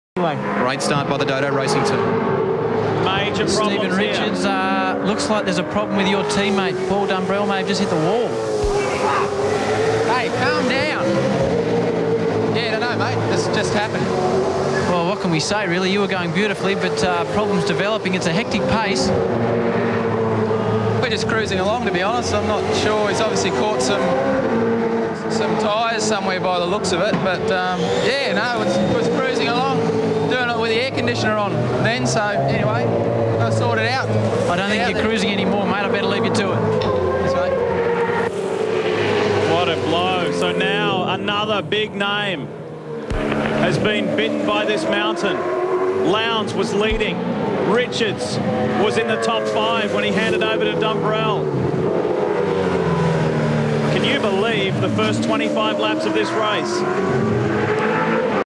2005 V8 Supercars Round10
Bathurst
Contact by Noske - very small. S Richards co-driver collects the wall with heavy damage. The team NOT impressed in the back ground!